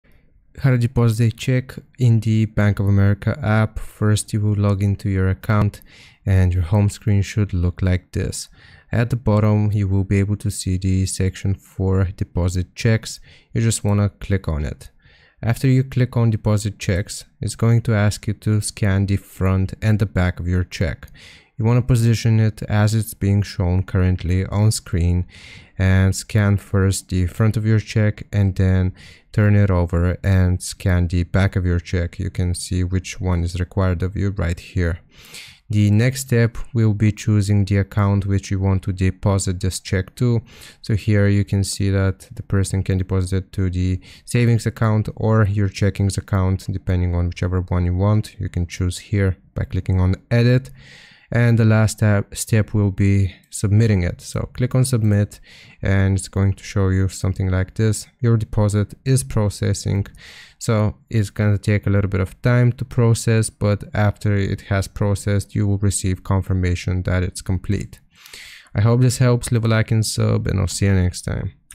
How To Deposit Check In Bank Of America App
In this video I show you how to deposit check in Bank of America app. If you want to learn how to deposit your cheque on the BofA mobile app then this is the tutorial for you.